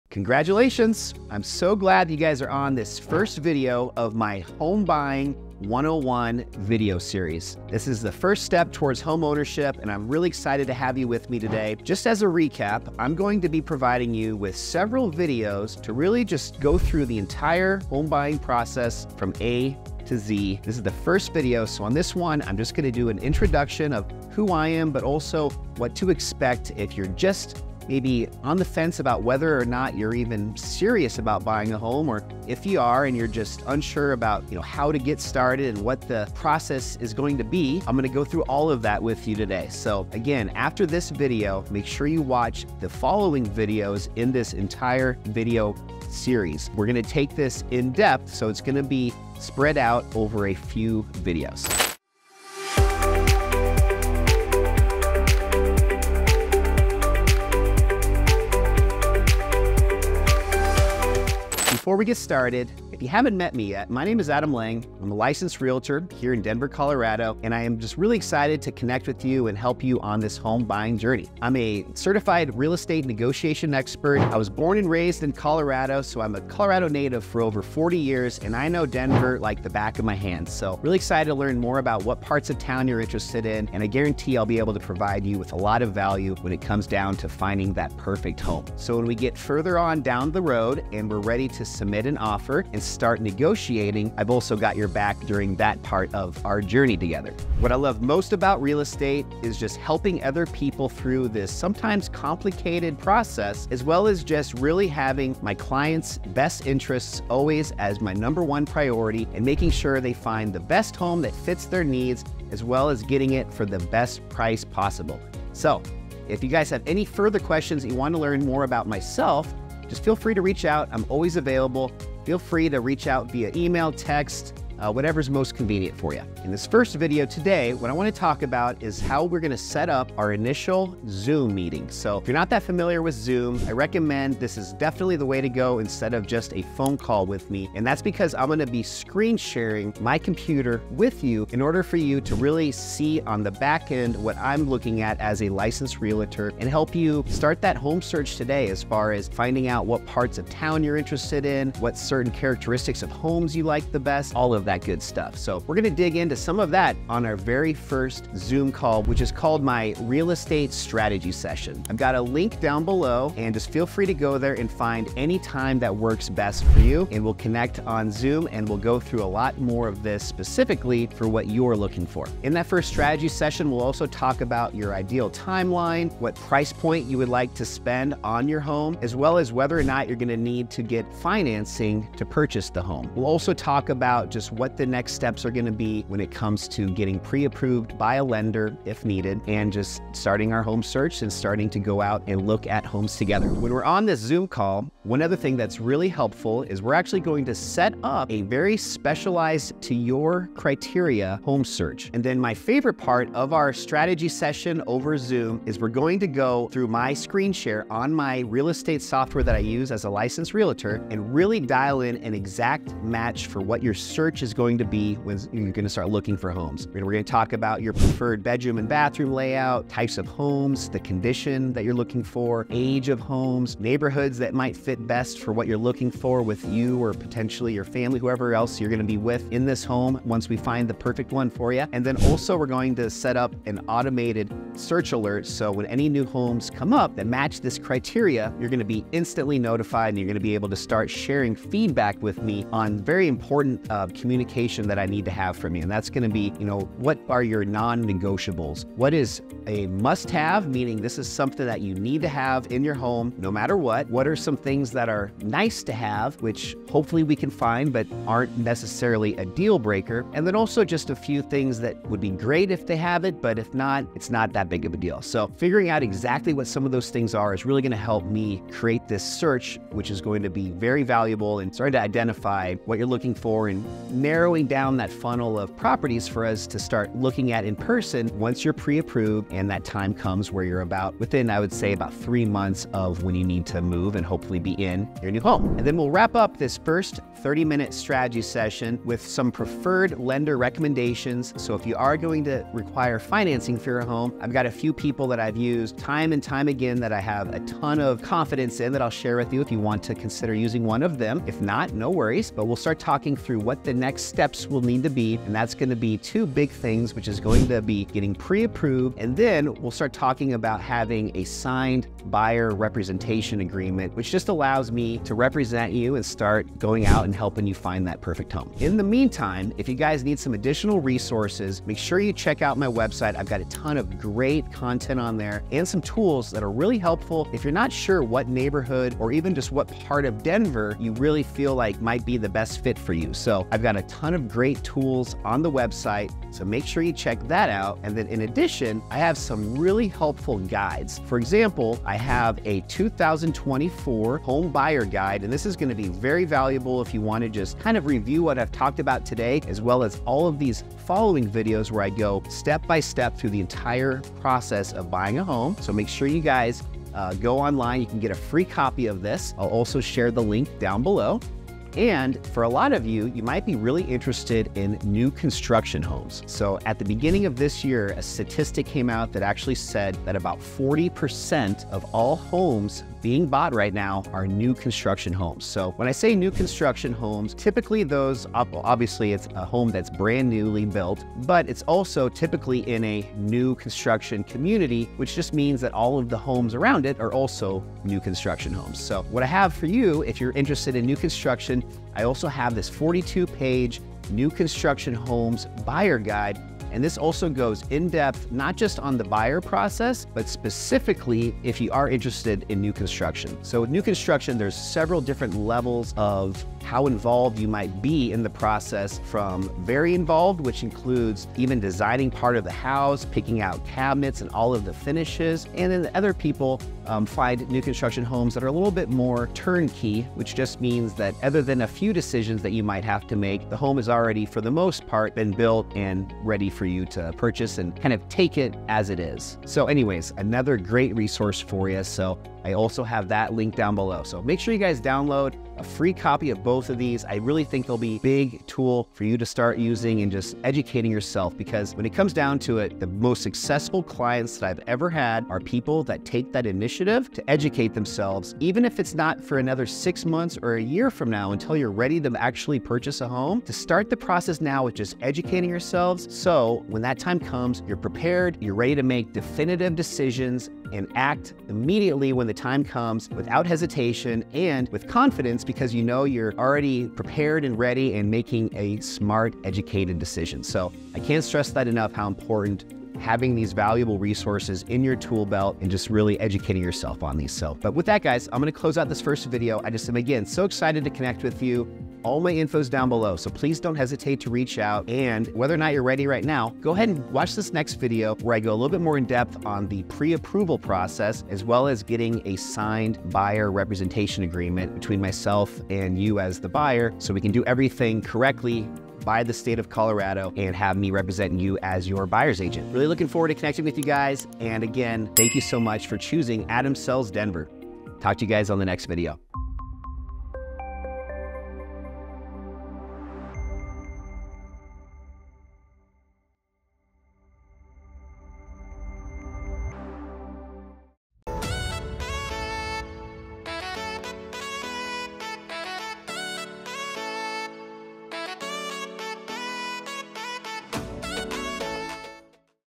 Homebuyer Video Series (1 of 5): Introduction from Adam Sells Denver
Welcome to the first video in my Homebuying A-Z Series! I’m Adam, and I’m excited to introduce myself, my team, and what you can expect from this series.

I’ll share a bit about my background as a realtor in Denver, my expertise in guiding homebuyers, and how my team at Adam Sells Denver is dedicated to helping you find your perfect home.

You’ll get to know the experienced professionals I work with, from agents to lenders and inspectors, all working together to ensure a smooth homebuying process.

What to Expect in This Series
Over the next 4 videos, I’ll be walking you through each step of buying a home, from the Buyer Agency Agreement and Pre-Approval, to the Home Search, making an offer, and finally getting you to Closing Day. My goal is to make this process as clear and simple as possible.

This series is designed to equip you with everything you need to confidently move forward in your homebuying journey.

Need personalized advice? Download my free Buyer’s Guide or schedule a 30-minute strategy session Let’s make your dream of homeownership a reality!